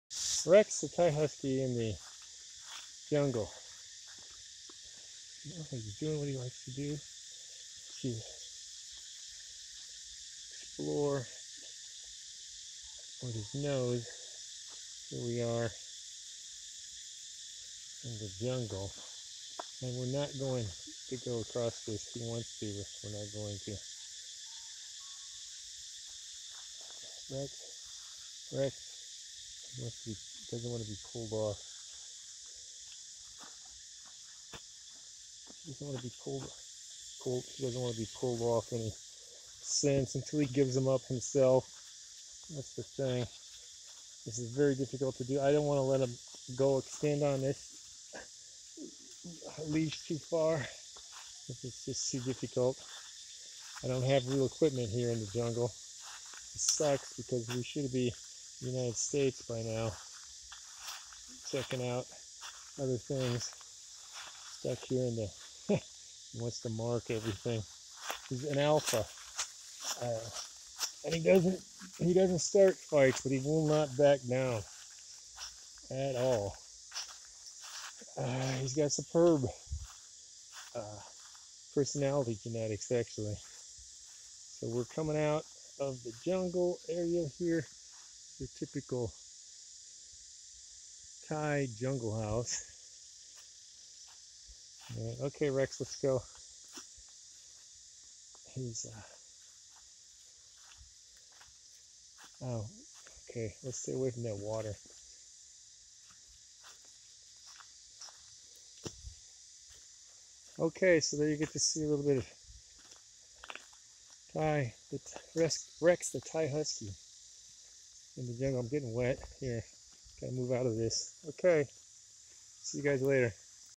Thai Husky in the Jungle, Rex, Clugston
RescueRex is in the blistering hot Jungles of Thailand.

Rex the Thai Husky shows you around. The jungle while Christophe Clugston supplies commentary for humans.